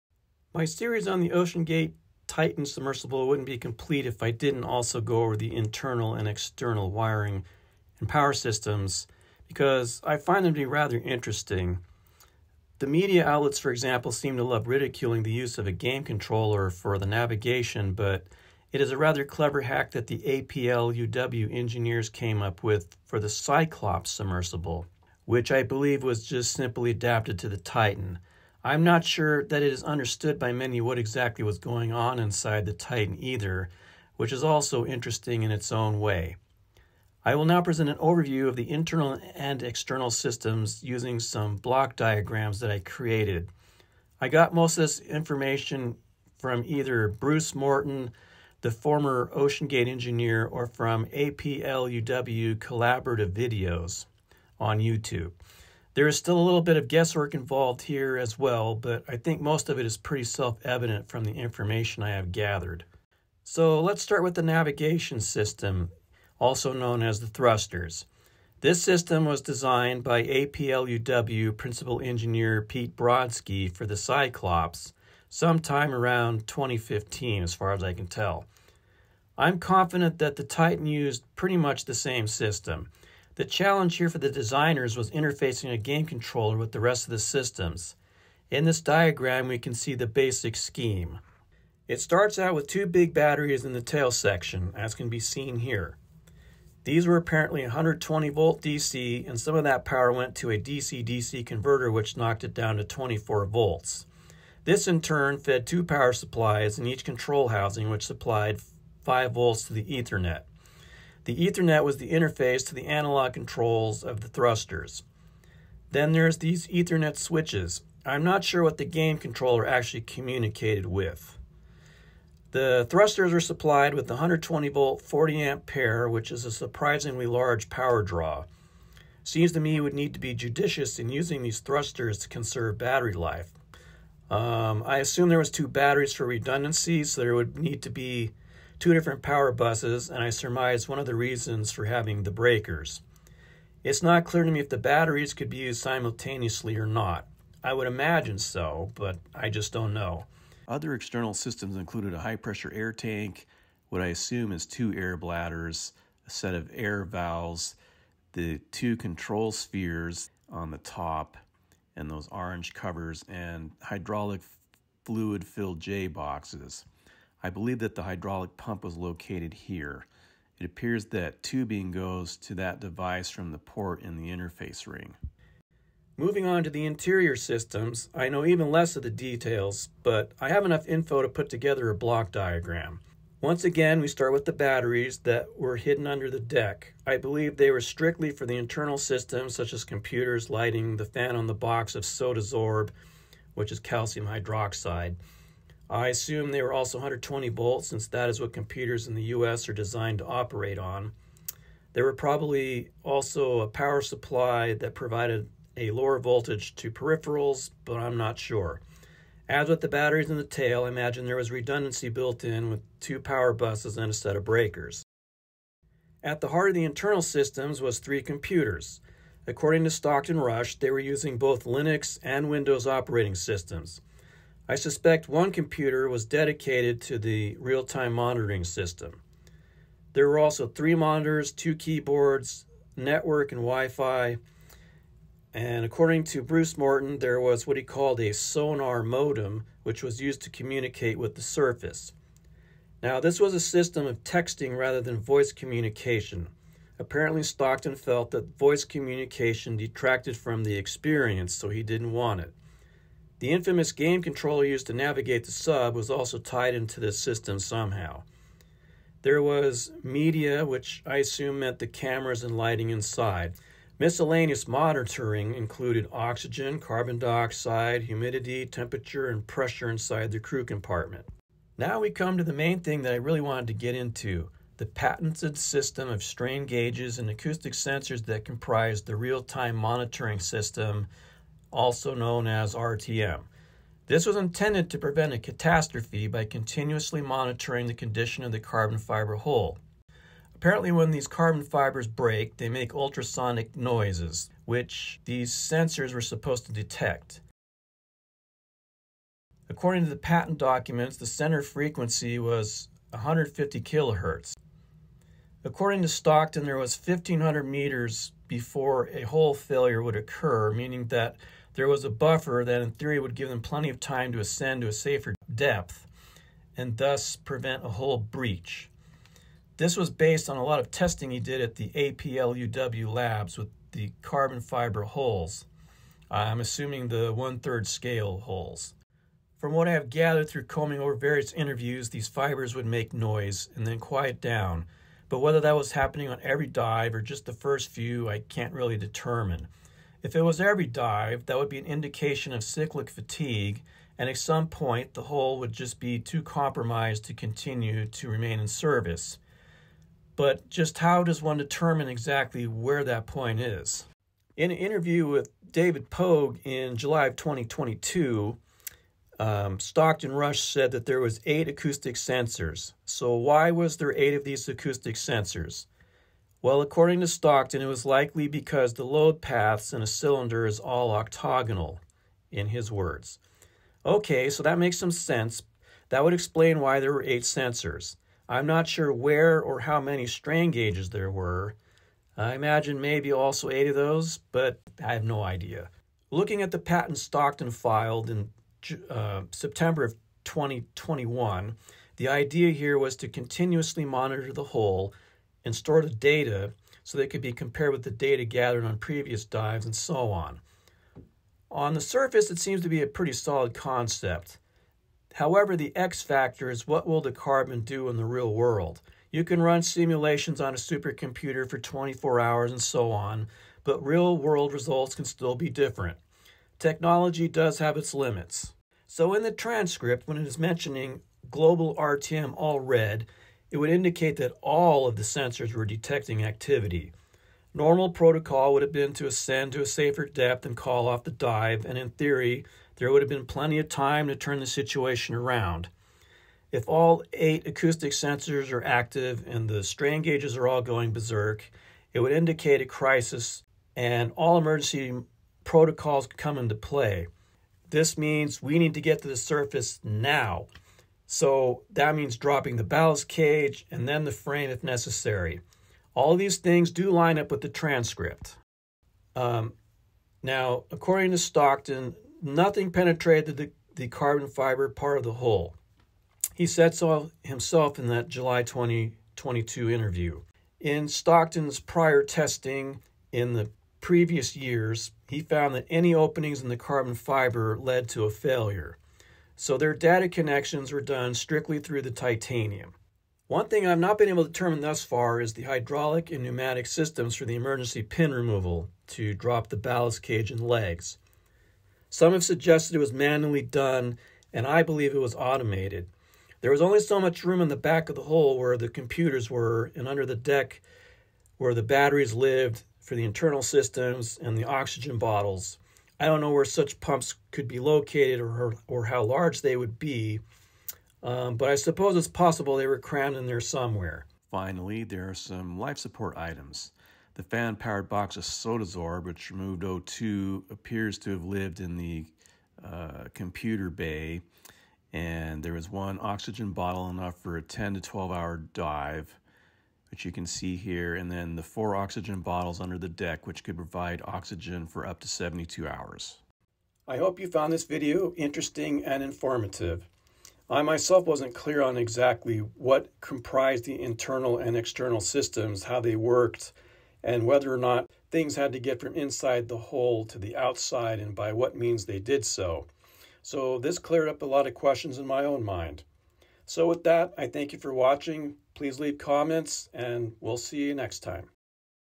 OceanGate Titan - INTERNAL & EXTERNAL SYSTEMS
UPDATE 9/18/2024: I had taken the OceanGate video series down because too much was based on the "leaked transcript", which I was never fully convinced was legitimate, and now that the hearings by the US Coast Guard and NTSB has began, it appears to now be proven a fake after viewing a 3D animation with the actual text communications. I am bringing some of these videos back by request.

An overview of how the thrusters worked, and of the internal systems with an emphasis on the Real Time Monitoring System (RTM). If you watch the part about the Real Time Monitoring system, you will see that this was thought out and tested, not just off the cuff use of carbon fiber with no regard to anyone's safety.

I made a mistake at 8:38 - Sodasorb obviously removes CO2 and not O2.

I discovered after I made the video that those big orange batteries in the tail were actually 148V and made by a company in Spain called "Ictineu". They weighed 242 lbs each. Here is a link to the data sheet